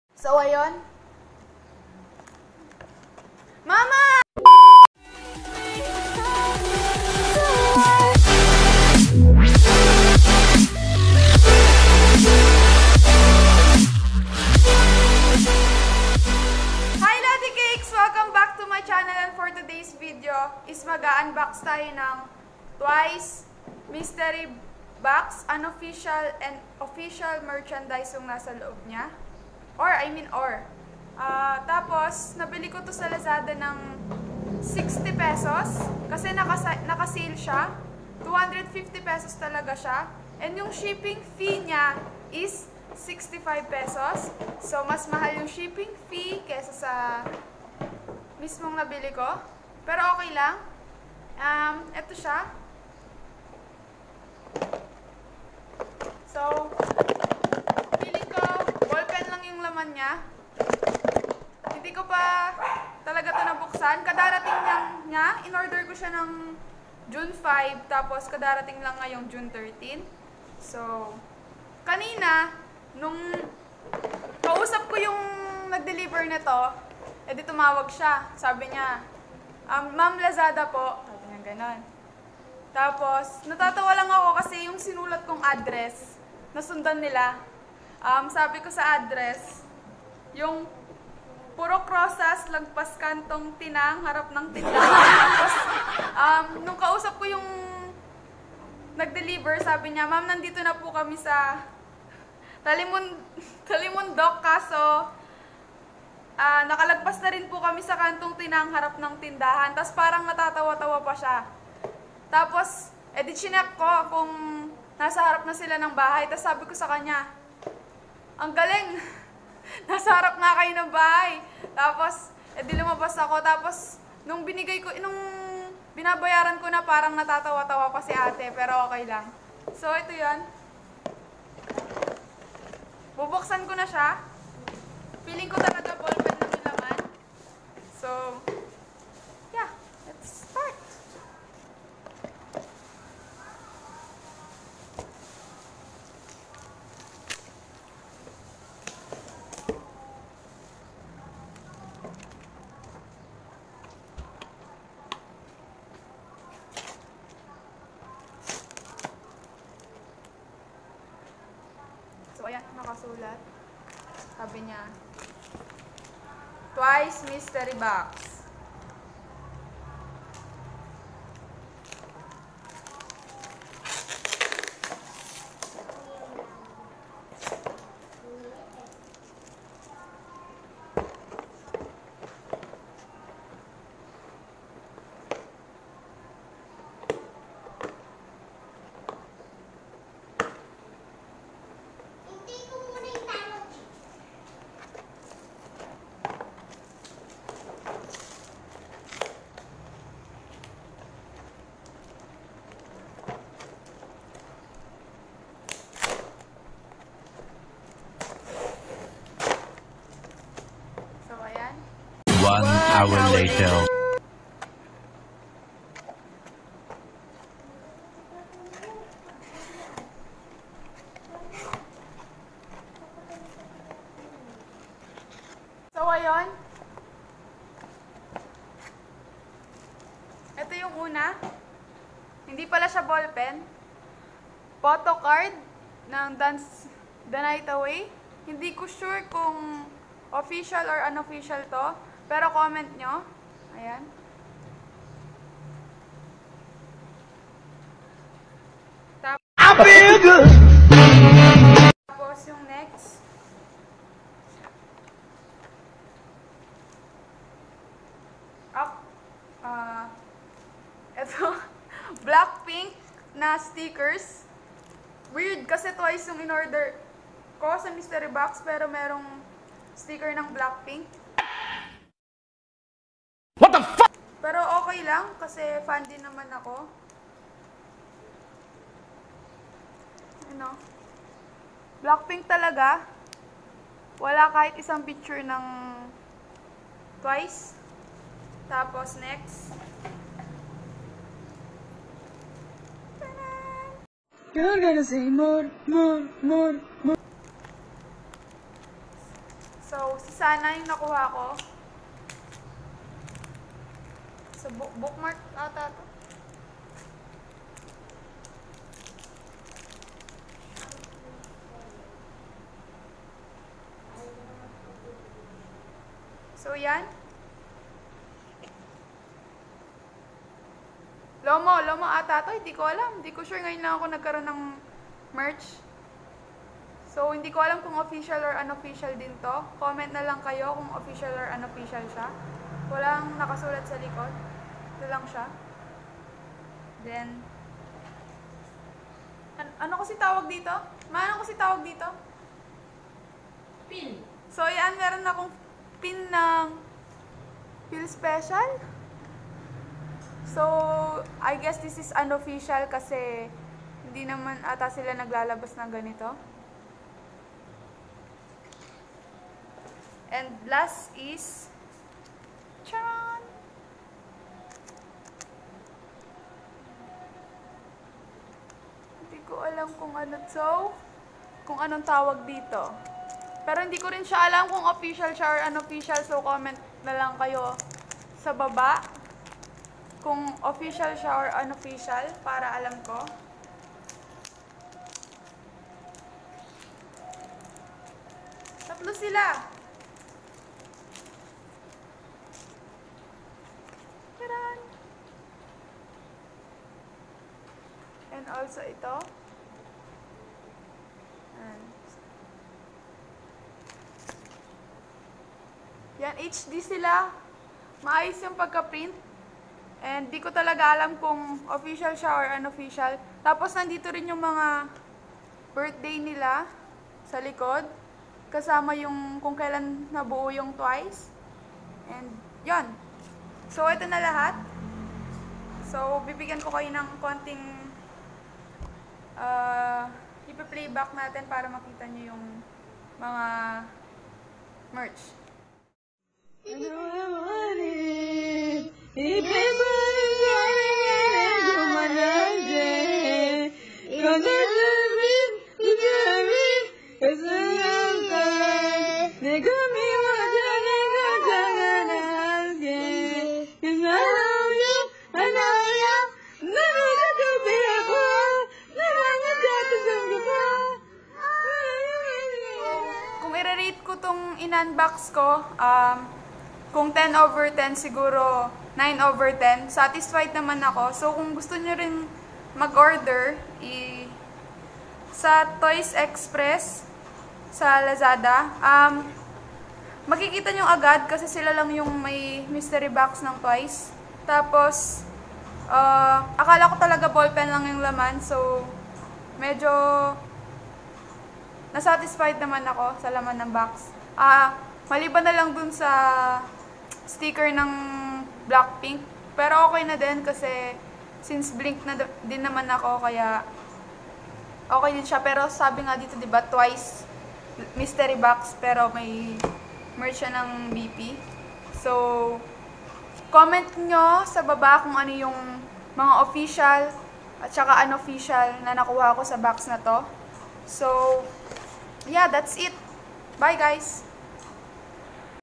UNBOXING TWICE OFFICIAL/UNOFFICIAL Merchandise (Mystery Box) | LAZADA.PH | PHP60
Hello Lodicakes! Please enjoy my UNBOXING#1 there will be lots of unboxing to come either about kpop or anything that i can unbox ssksksjsk ! I'm a once and it's my first time to buy some twice merchandise!!

I got the mystery box at : Lazada.PH (Toys Express) or just search "Twice Mystery Box" .. Only Toys Express offers Twice Mystery Box and I bet you'll find the Mystery Box easy.🍭